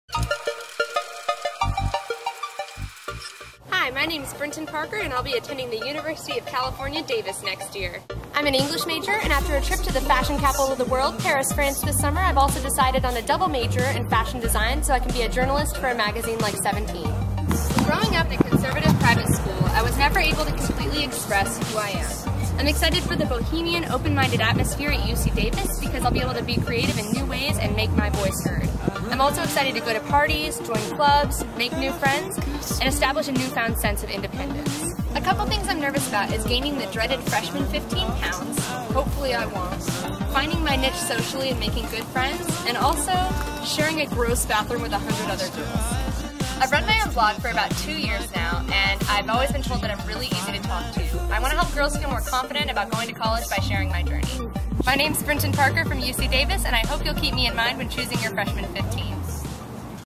Seventeen Magazine "Freshman 15" Audition
Hi, I'm Brinton from beautiful California, and I'll be going to UC Davis (go Aggies!) in the fall. I was lucky enough to be selected for the second round of the Seventeen Magazine "Freshman 15" selection process, and this is my audition video. Wish me luck!

Thank you, Seventeen, for giving me this wonderful opportunity and considering me in your selection.